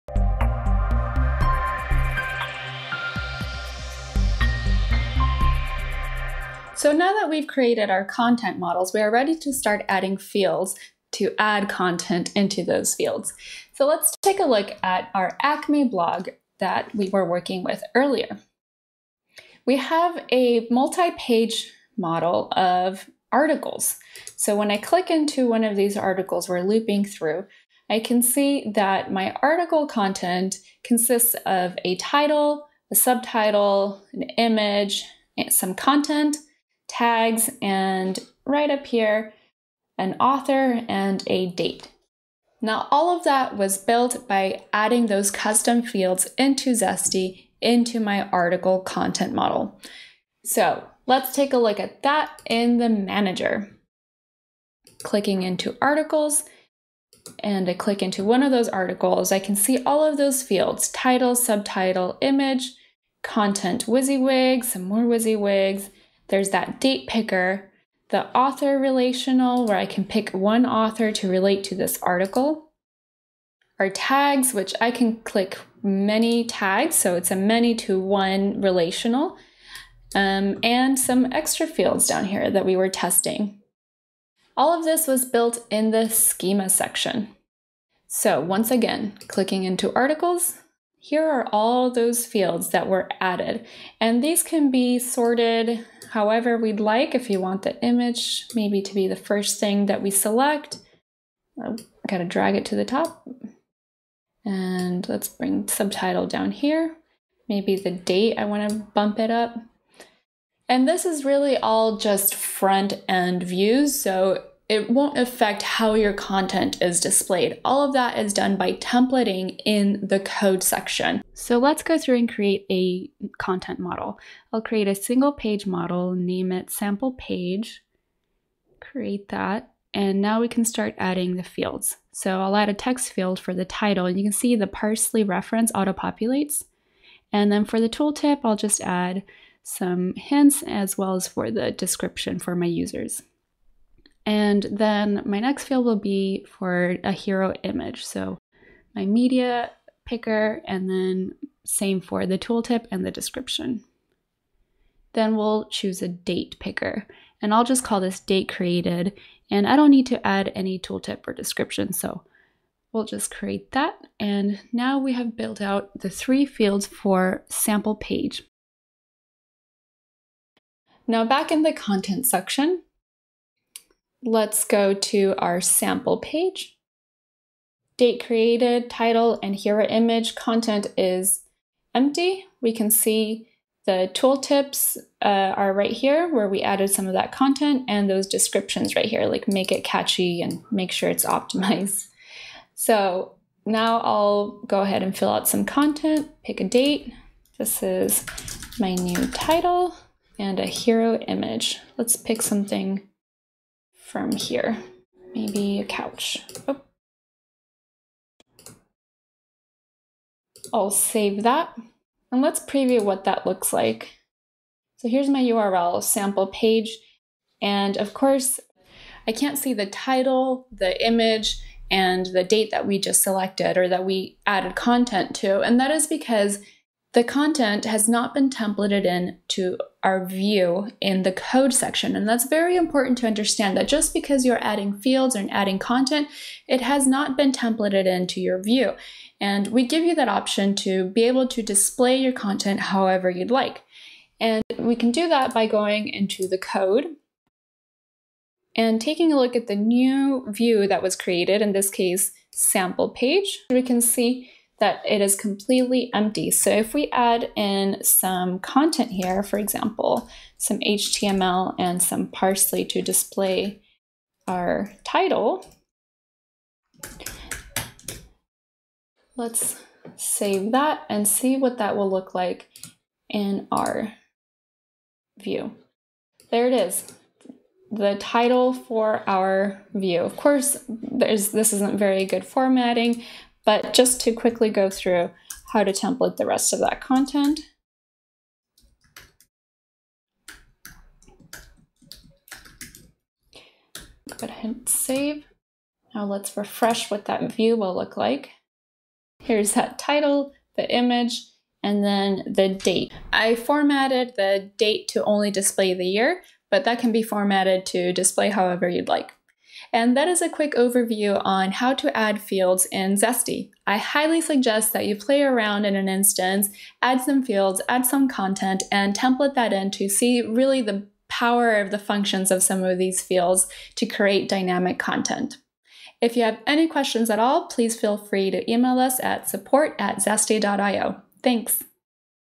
Adding fields for content in Zesty.io
A quick introduction to Fields in Zesty.io to help you get started in building your custom CMS experience. Fields are used to create dynamic content and you can reference them throughout your website using Web Engine or through endpoints. All fields and their content are completely headless and API accessible!